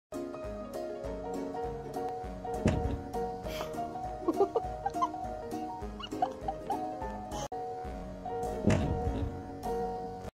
Cat Jumps Face First into Glass
Kitten jumps into window.